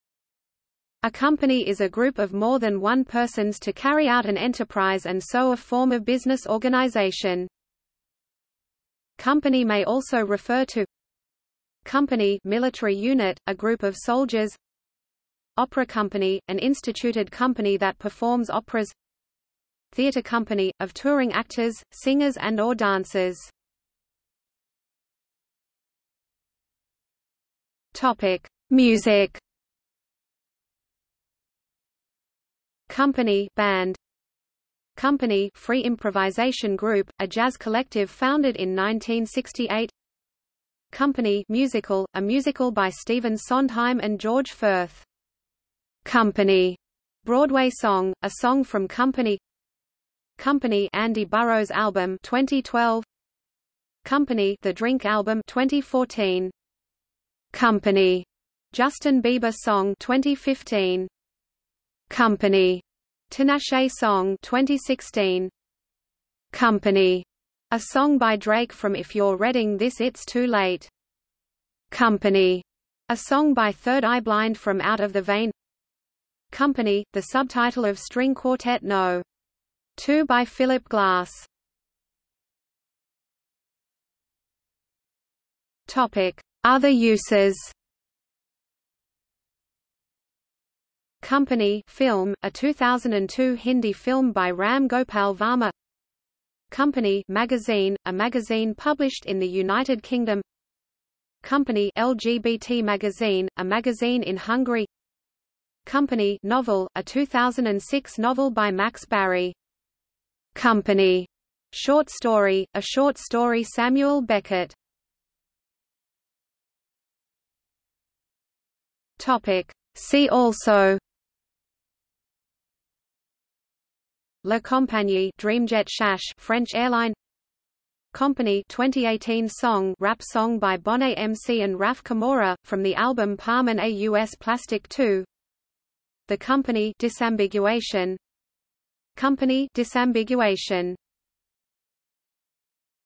Company (disambiguation) | Wikipedia audio article
00:00:24 Music
00:01:22 Other uses
00:01:55 See also

- Socrates

SUMMARY
A company is a group of more than one persons to carry out an enterprise and so a form of business organization.
Company may also refer to:

Company (military unit), a group of soldiers
Opera company, an instituted company that performs operas
Theatre company, of touring actors, singers and/or dancers